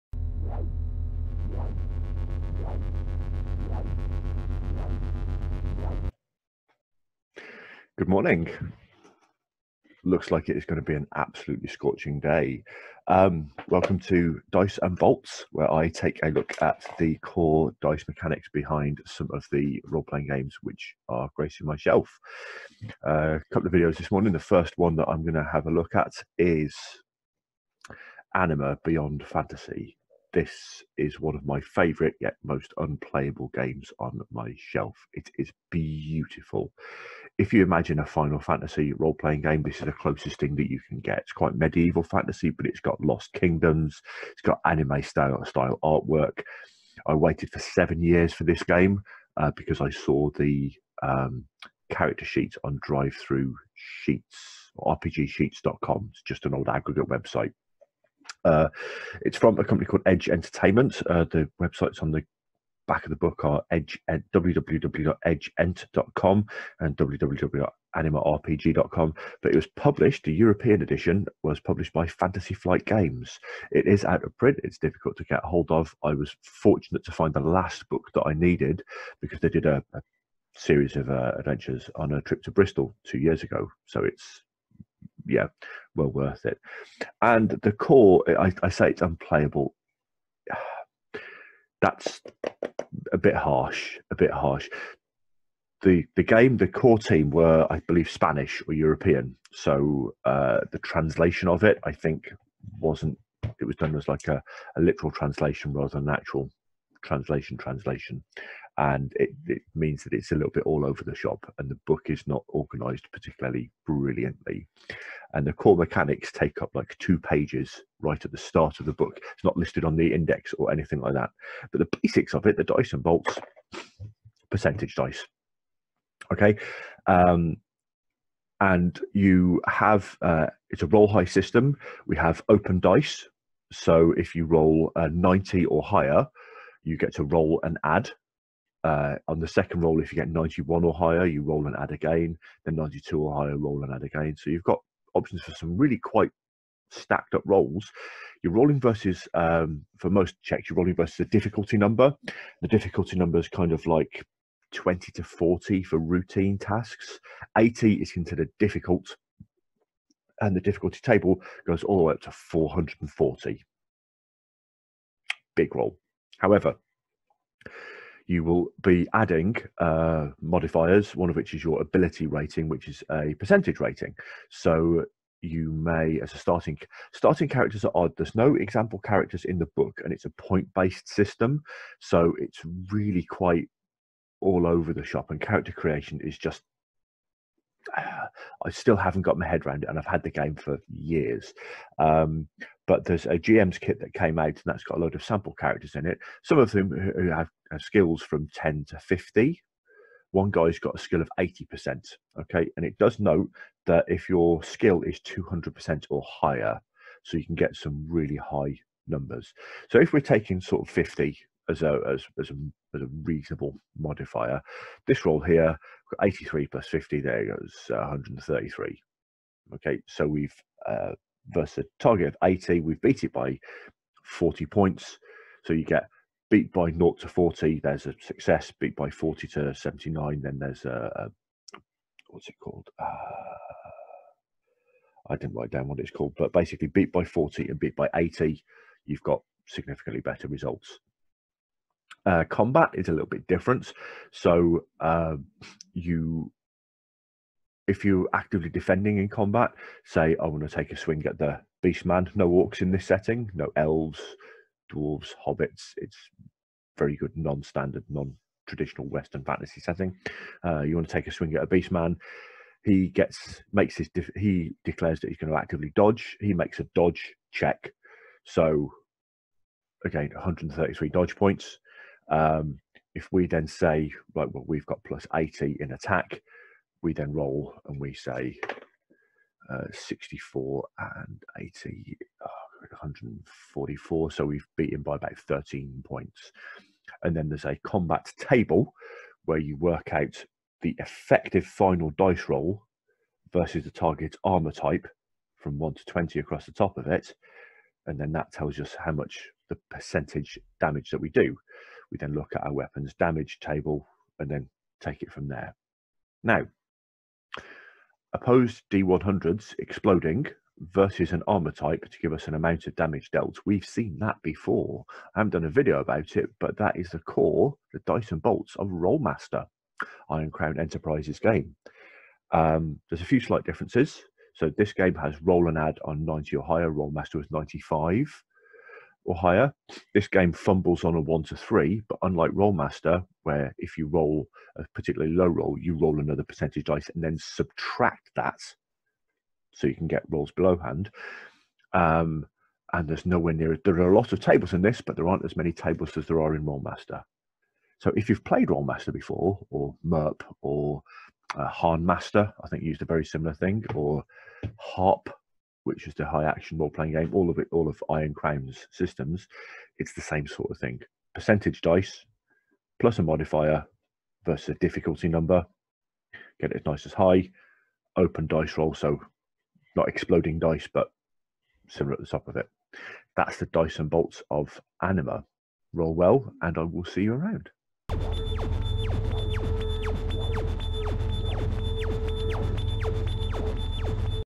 Dice and Bolts 21 - Anima
the percentage based system behind the wild game Anima: Beyond Fantasy.